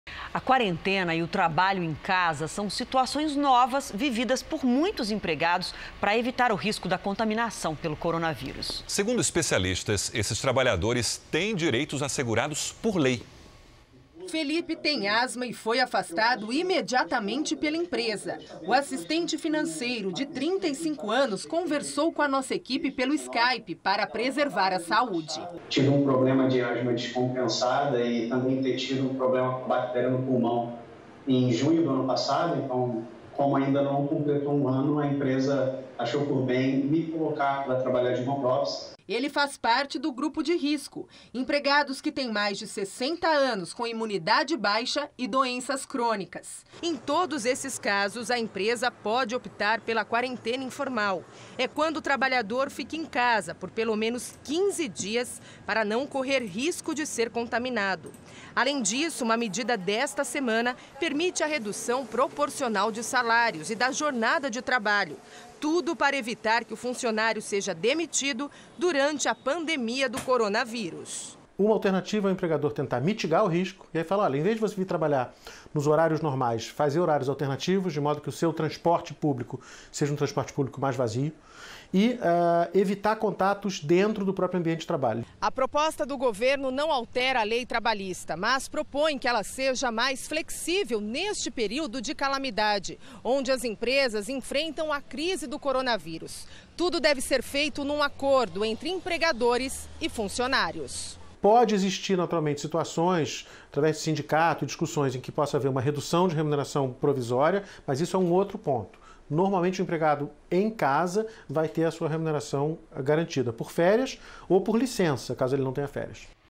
Trabalhadores em quarentena têm direitos assegurados por lei, dizem especialistas
Uma medida desta semana permite a redução proporcional de salários e da jornada de trabalho como forma de evitar que o funcionário seja demitido durante a pandemia de coronavírus. A proposta do governo, porém, não altera a lei trabalhista. Entenda na reportagem.